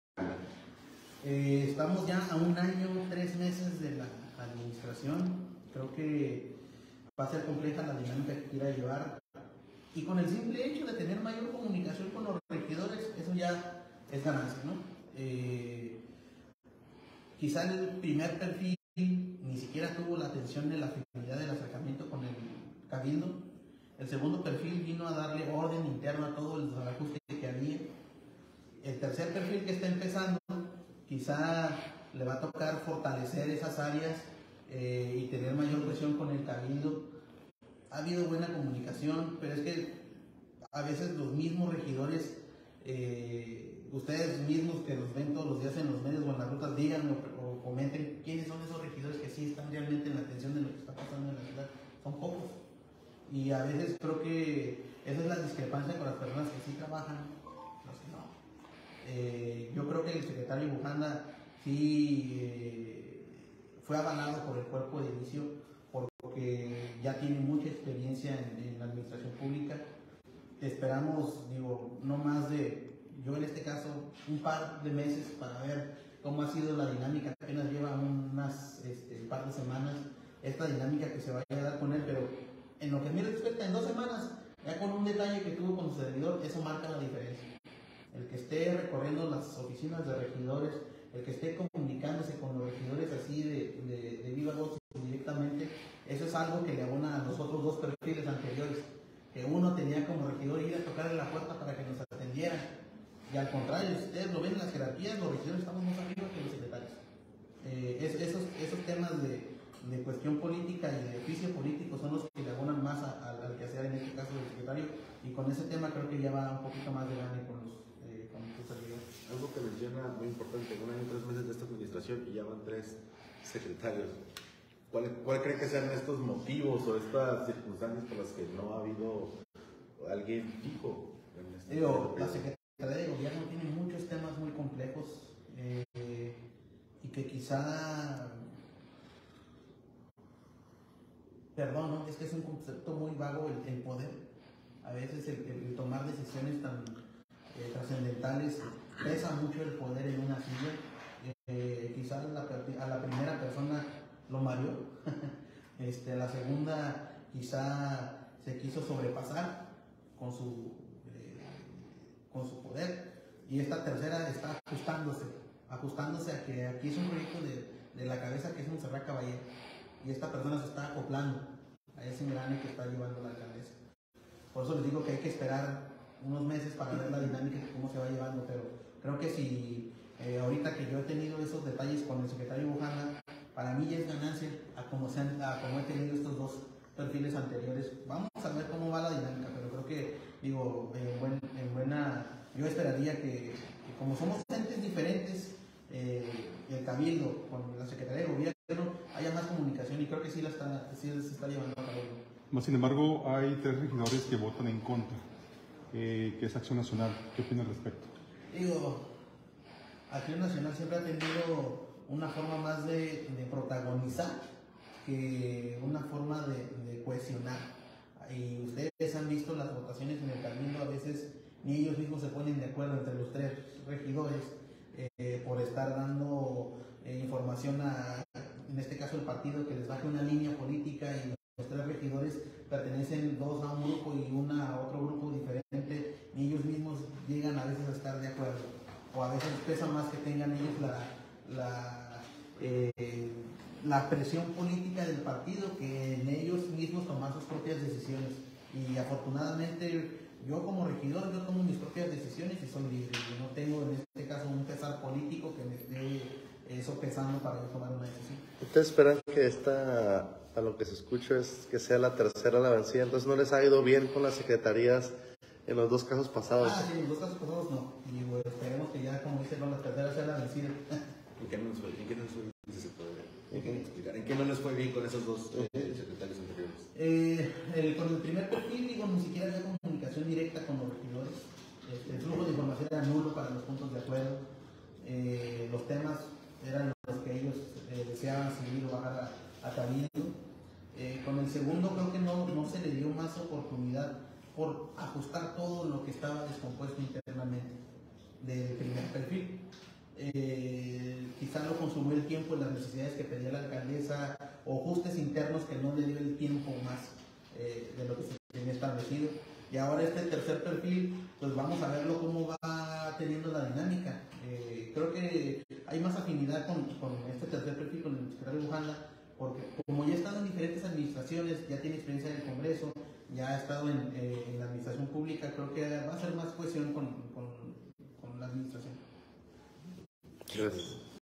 Entrevista con. El Regidos Edgar Montiel de la Comisión de Derchos Humanos, Migrantes y grupos
Entrevista con. El Regidos Edgar Montiel de la Comisión de Derchos Humanos, Migrantes y grupos indígenas